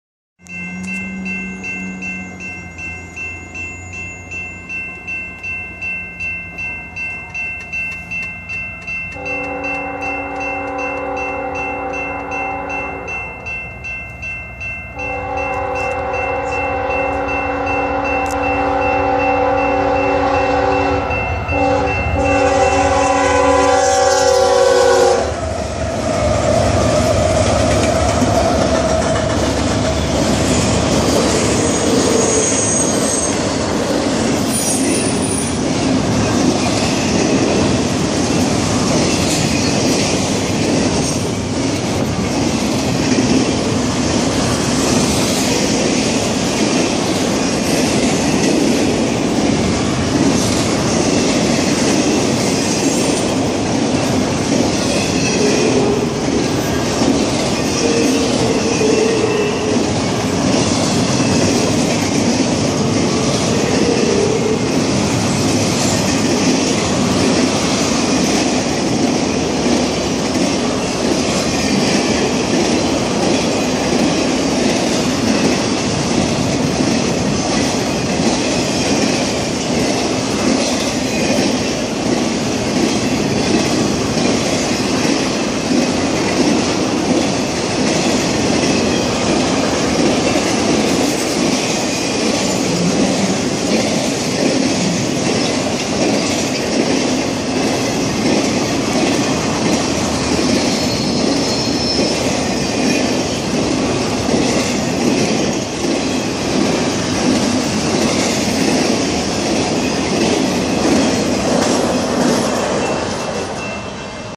BNSF 6764 West, Lockport illinois
BNSF ES44DC 6764 Leads a Z train through Lockport illinois on the BNSF Chillicothe subdivision. That horn seems a little fouled huh? I was with EJE665 when this came.
Taken 3/18/16